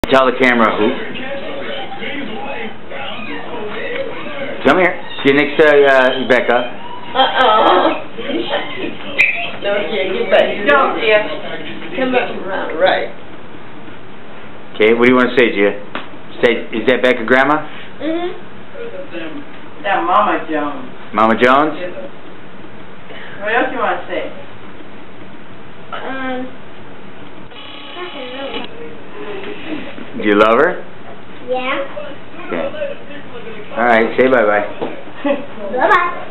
Gia,...with Becca Jones
Mama jones is loved dearly by Gia, Becca has saved dozens of children in Oakland, and Gia loves her very much.